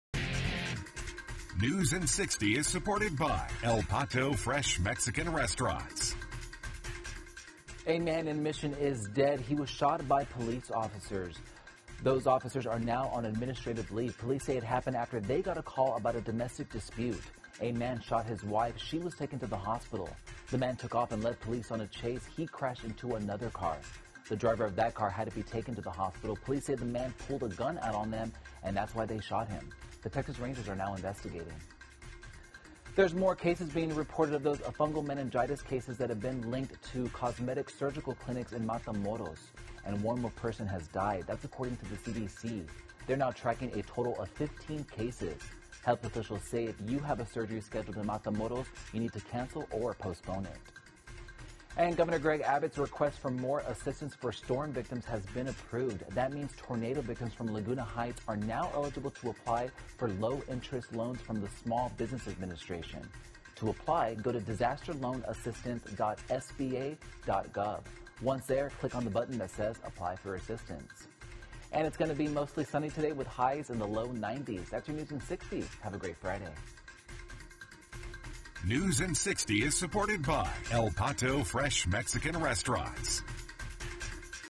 KRGV CHANNEL 5 NEWS Update - May 26
Good morning. A man in Mission is dead after being shot by police, cases of Fungal Meningitis are on the rise and Governor Abbott's request for additional storm assistance for tornado victims has been approved. Mostly sunny today and that is your News in 60. Have a great weekend.